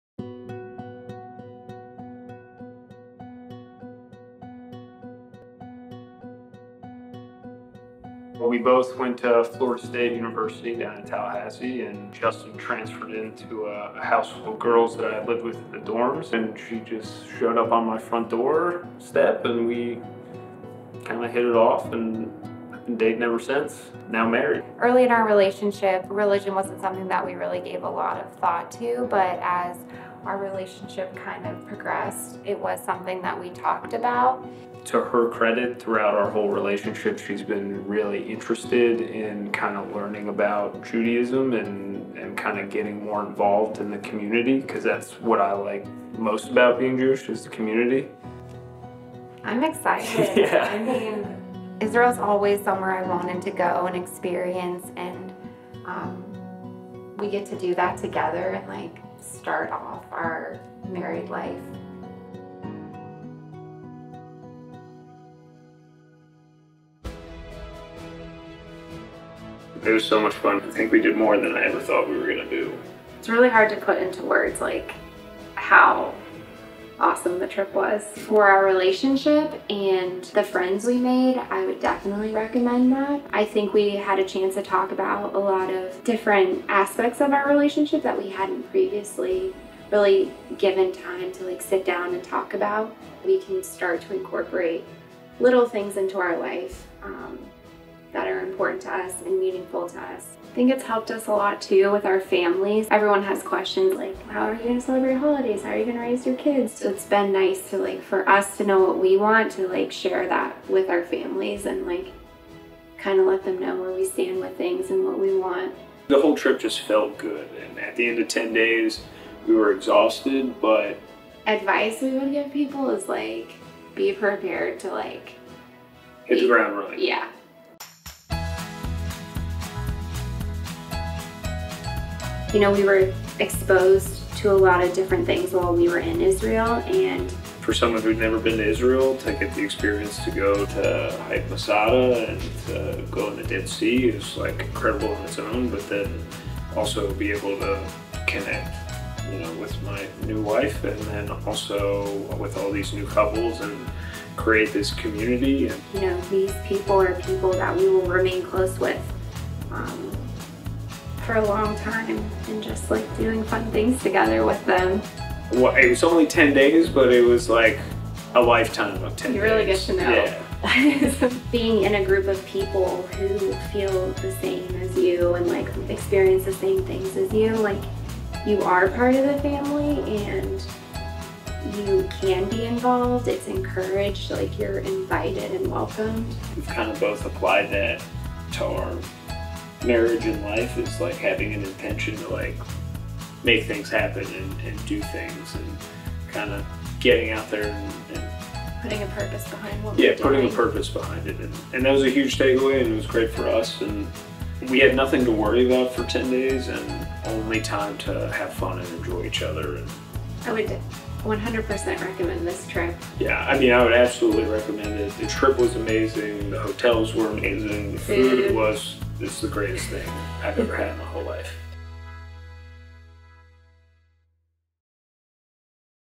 Honeymoon Israel Pittsburgh!
Honeymoon Israel Pittsburgh is back for it's second trip: Jan 16-26, 2020! Explore Israel with your partner and other couples from Pittsburgh!

Curious to learn more about HoneymoonIsrael?? Take it straight from Justinn and Aaron who attended Pittsburgh's first Honeymoon Israel cohort!

Questions?